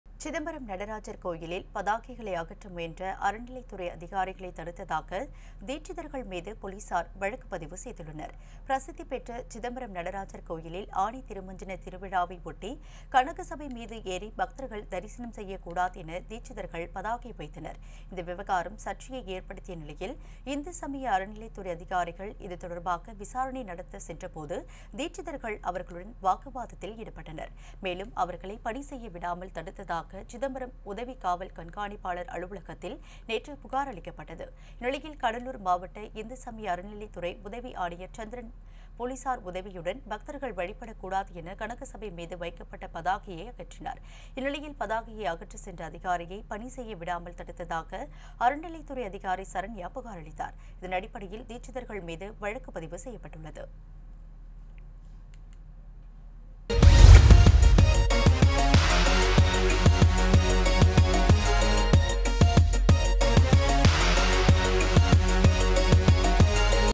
நடராஜர் கோயில் தீட்சிதர்கள் மீது போலீஸார் வழக்குப்பதிவு! | Chidambaram | Dikshitar | Police
சிதம்பரம் நடராஜர் கோயிலில் பதாகைகளை அகற்ற முயன்ற அறநிலையத்துறை அதிகாரிகளைத் தடுத்ததாக தீட்சிதர்கள் மீது போலீஸார் வழக்குப்பதிவு செய்துள்ளனர்.